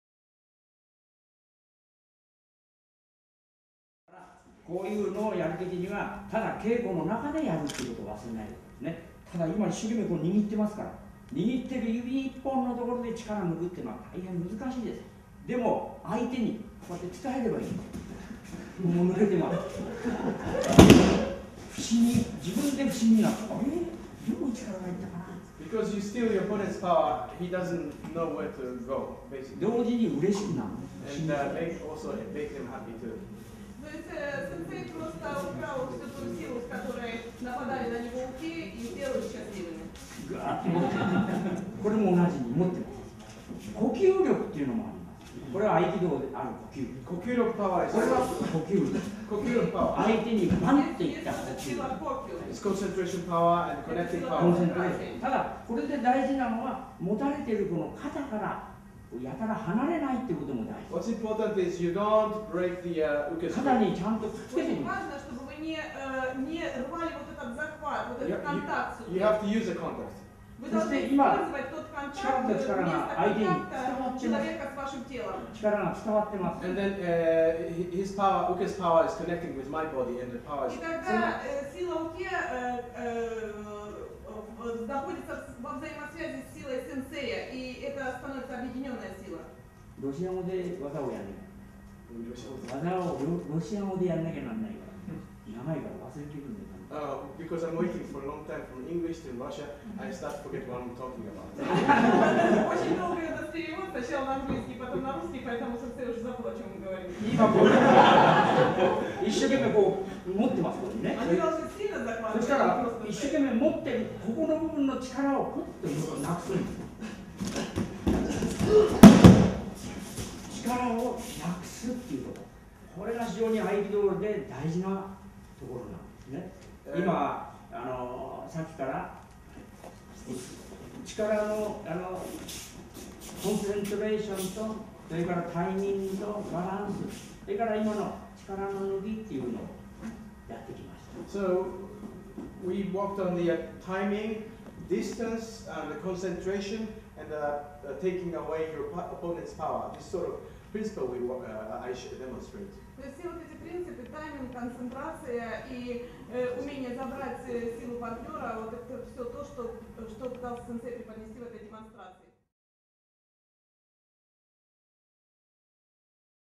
Aikido techniques demonstration by Sensei Kyoichi Inoue in Misogikan Dojo. 23-12-2007. Part 3
Aikido techniques demonstration by Sensei Kyouichi Inoue  (9-th dan Yoshinkan Aikido, Hanshi) during the grand opening of a new Misogikan Aikido Honbu Dojo, Moscow, Russia.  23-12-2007. Part 3.

Демонстрация техник айкидо сэнсэем Кёити Иноуэ (9-ый дан Ёсинкан айкидо, Ханси) во время торжественного открытия нового хомбу додзё школы айкидо Мисогикан. Часть 3.
Москва, Россия, 23 декабря 2007 г.